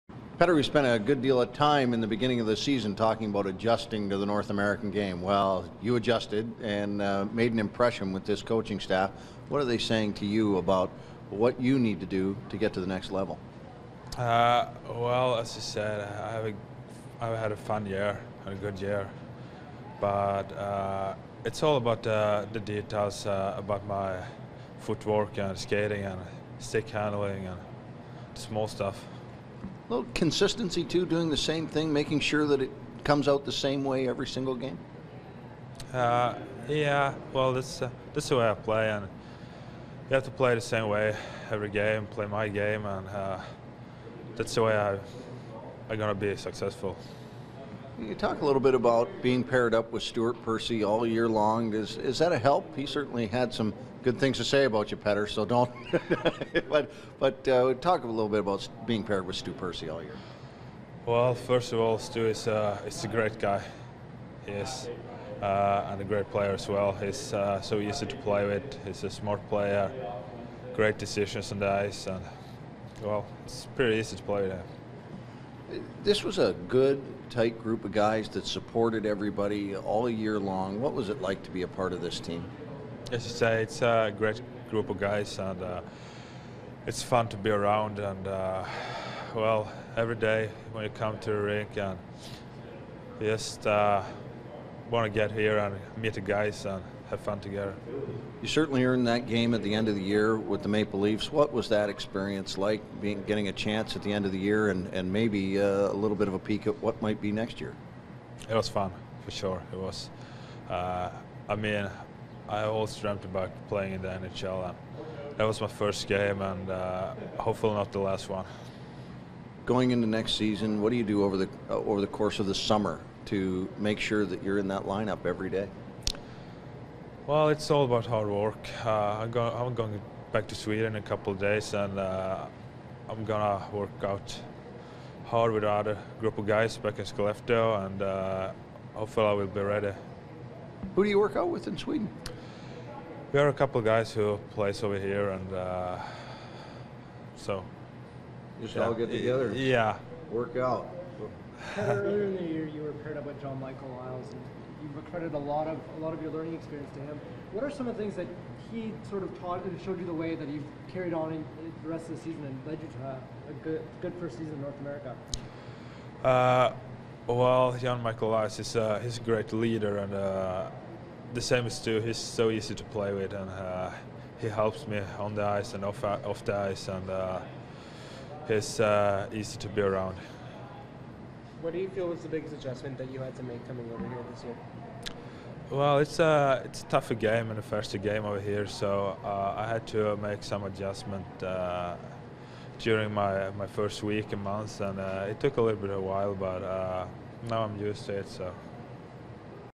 Petter Granberg on Locker Clean Out Day - June 2014
Petter Granberg speaks with Leafs TV on Marlies Locker Clean Out Day.